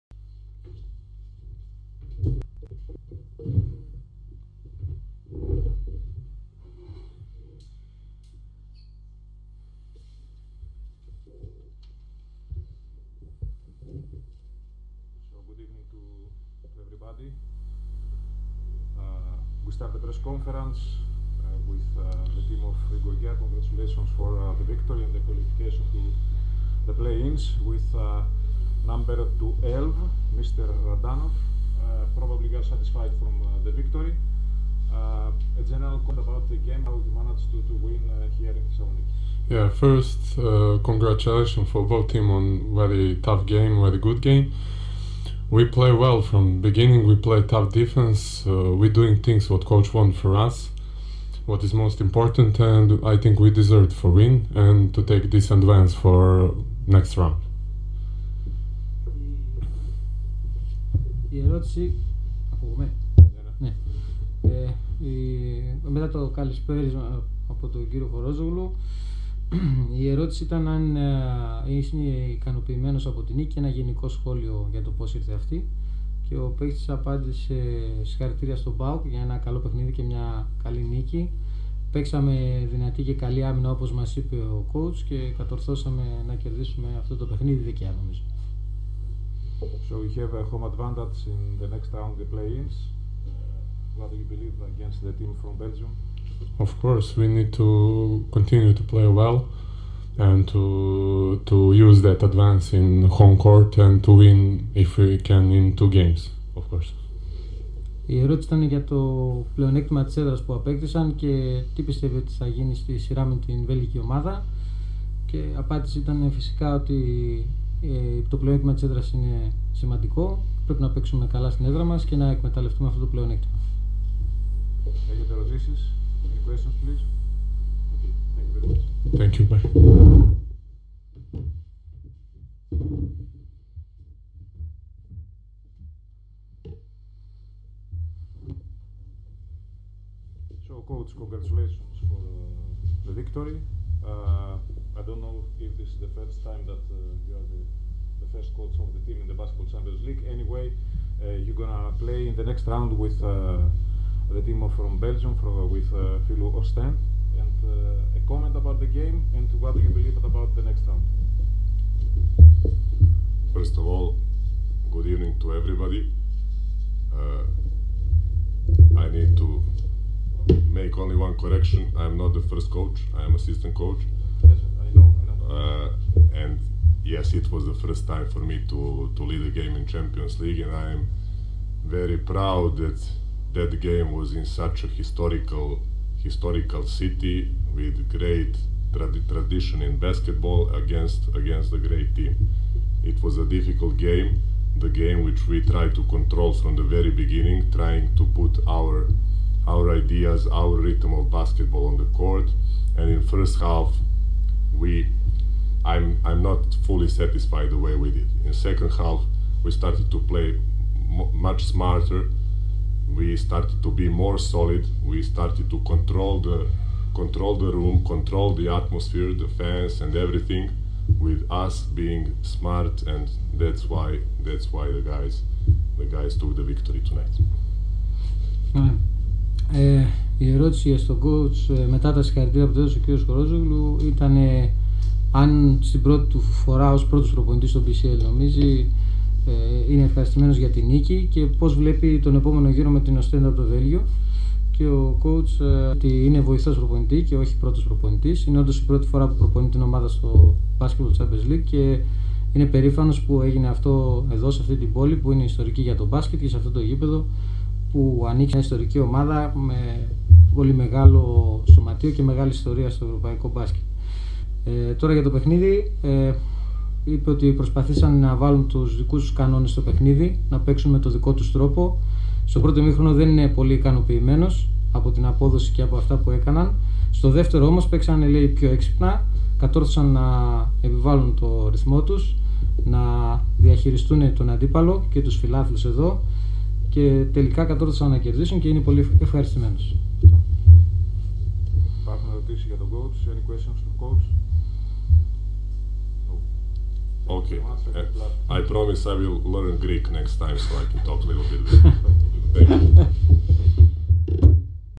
Izjave Alekse Radanova i Marka Baraća nakon pobjede nad PAOK-om
U poslednjem kolu grupne faze Lige šampiona, Igokea je kao gost savladala PAOK sa 75-64.